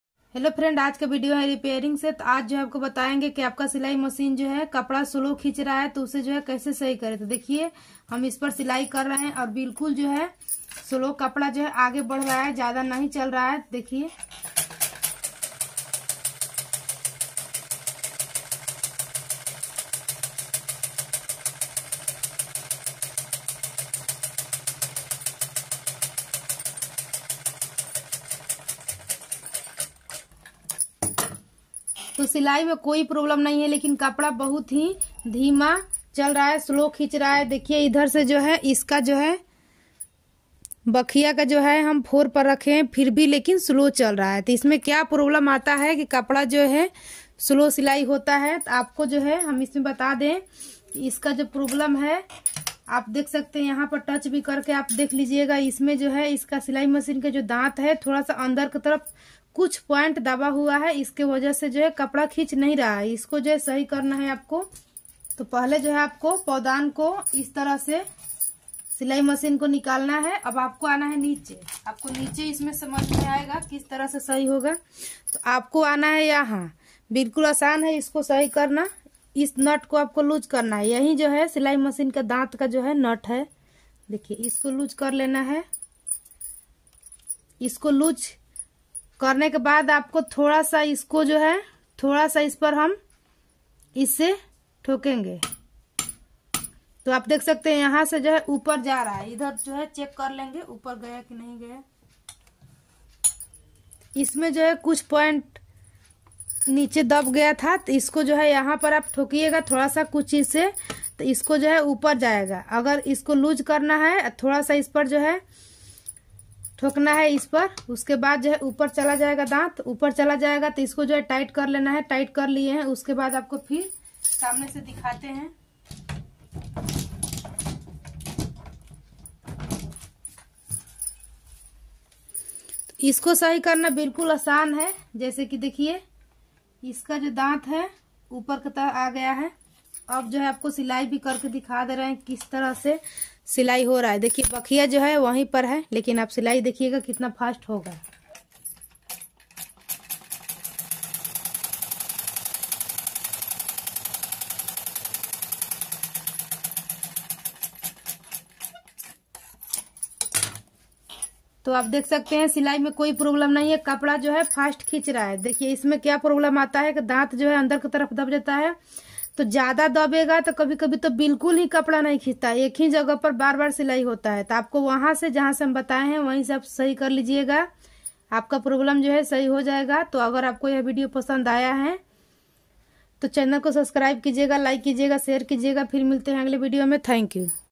silai machine repairing।सिलाई मशीन कपड़ा नही खींच रही हैं।sewing machine repairing।
About video: इस विडियो में बताया गया है कि जब सिलाई मशीन कपड़ा नहीं खींच  रही है तो कैसे ठीक करें।